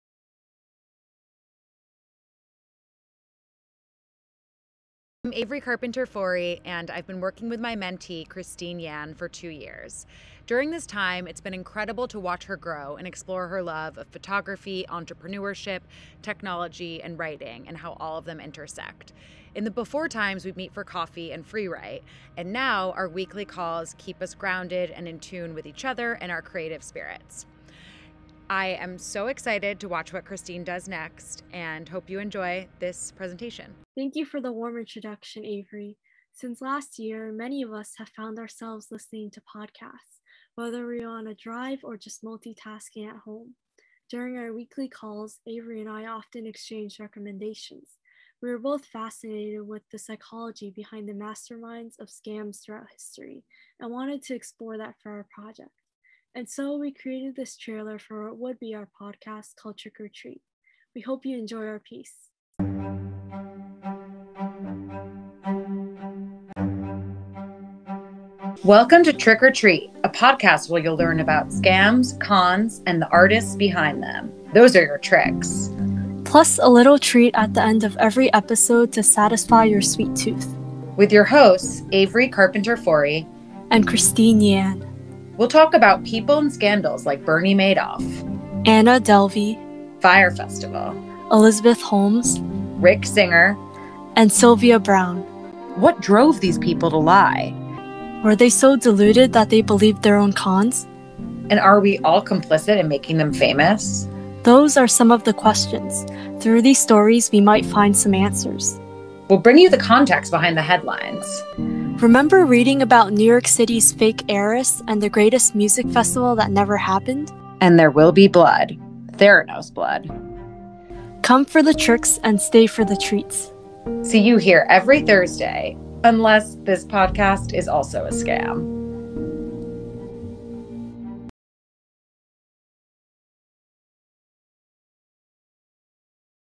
Christine Yan & Avery Carpenter Forrey, ‘Trick or Treat: The Trailer’
Mentee Christine Yan & Mentor Avery Carpenter Forrey perform ‘Trick or Treat: The Trailer’ at the May 21, 2021 virtual episode of Girls Write Now Live: Unmuted.